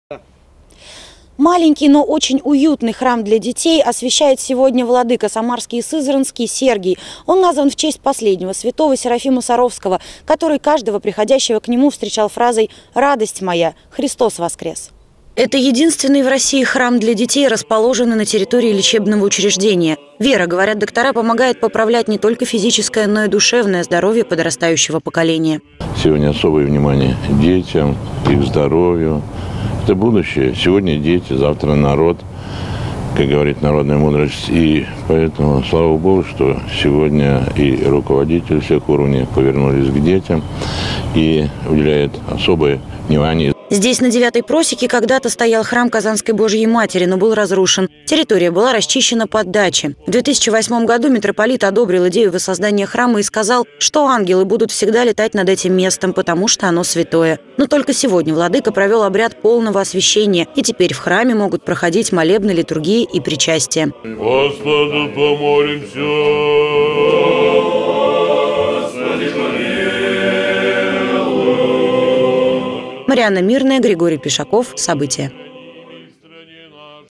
В Самаре освятили храм Серафима Саровского
Долгая дорога к освящению храма. Церковь  Серафима Саровского,  построенная  еще в 2008 году, теперь может вести полноценную службу.  Митрополит Самарский и Сызранский  Сергий сегодня  освятил  храм. Его главные прихожане - дети, которые находятся на лечении.